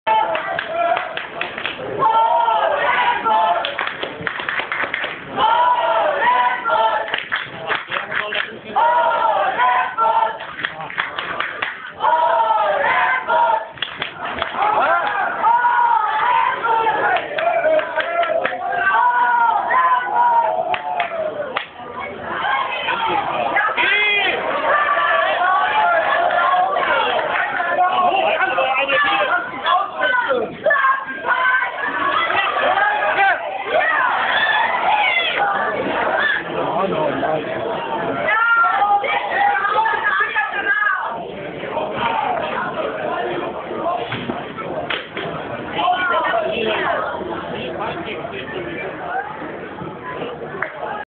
SV 10 Hohenlimburg - SV Rothemühle (4)
Spiel SV 10 Hohenlimburg - SV Rothemühle.
Für Hohenlimburg ging es um denn aufstieg hätten sie gewonnen wären sie in der Verbandsliga so sind sie aber nur 3. geworden. Hohenlimburg hat 2:3 verloren.
Kein schöner tag =(